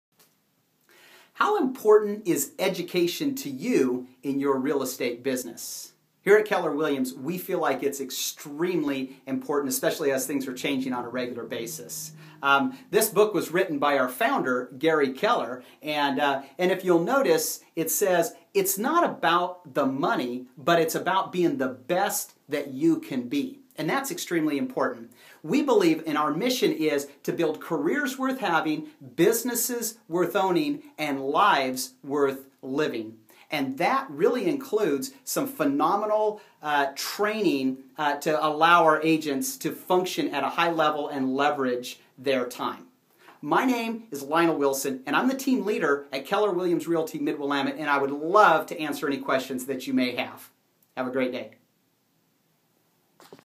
Real Estate Training and Education with Keller Williams Realty
How important is training in your real estate career? Do you have a plan and a system to grow your business at a high level? At Keller Williams Realty we believe in training and provide it on a regular basis. If you are considering a change in your career be sure and contact Lionel Wilson the team leader who would love to have a conversation with you.

Keller Williams Realty Mid-Willamette has an office in Corvallis, Albany and Lebanon. our mission to to help our agents "Build a career with having, businesses worth owning and lives worth living.